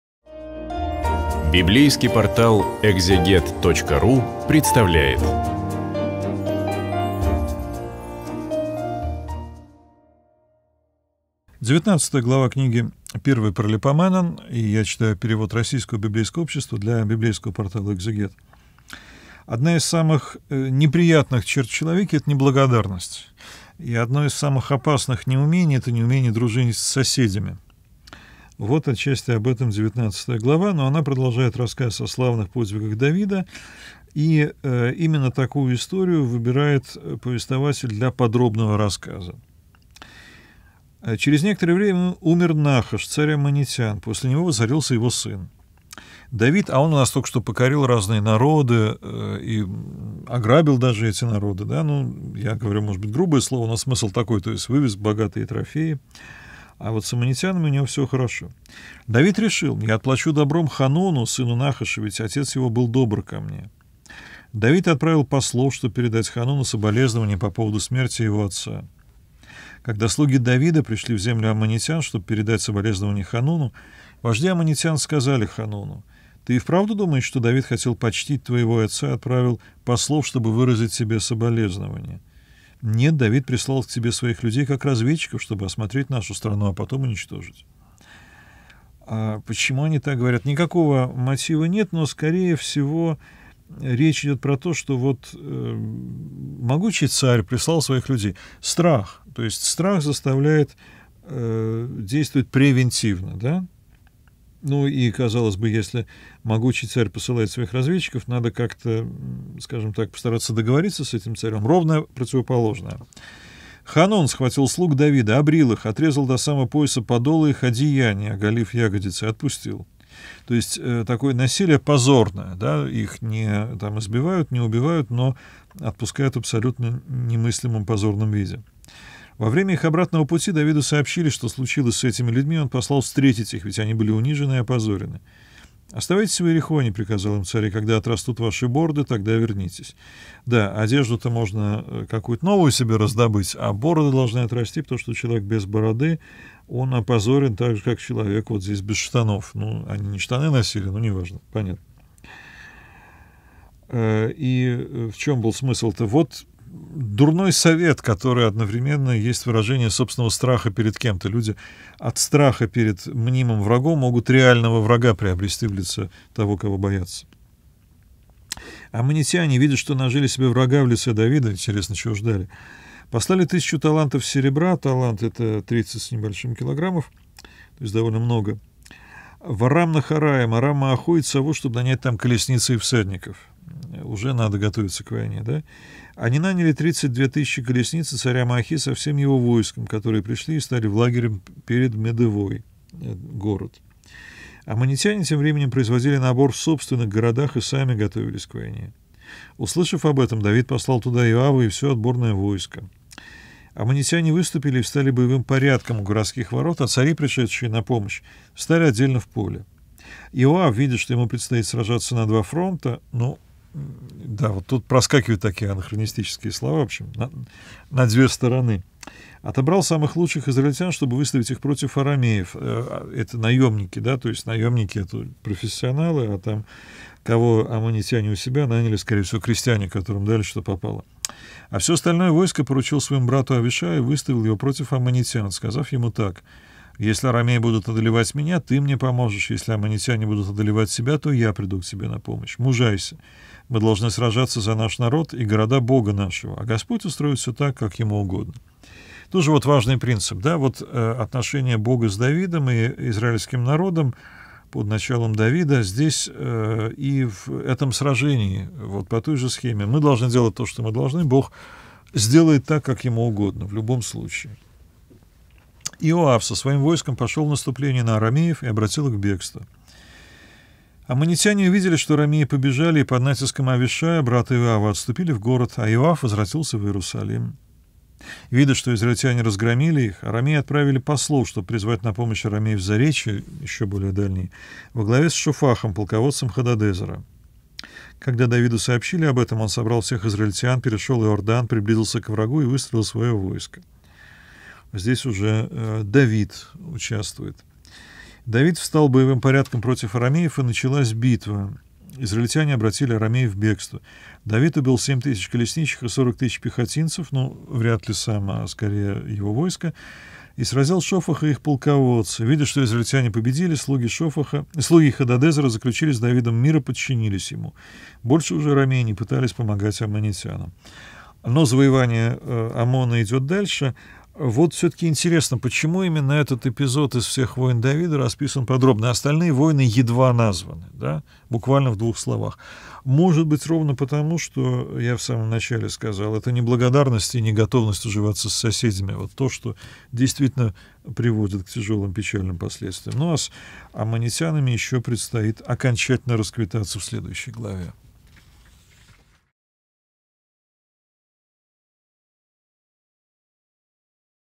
1-я Книга Паралипоменон. Глава 19. Андрей Десницкий. Ветхий Завет
В 19-ой главе автор повествует о том, как Давид, узнав о смерти  Нааса, Аммонитского царя, решил почтить его и отправил своих слуг к его сыну, дабы выразить соболезнования. Однако слуги Аннона, сына царя Нааса, боясь израильтян, оклеветали людей Давида и опозорили их. За это пошёл Давид на войну с аммонитянами и арамеями, которые решились помогать первым. Арамеи были разбиты.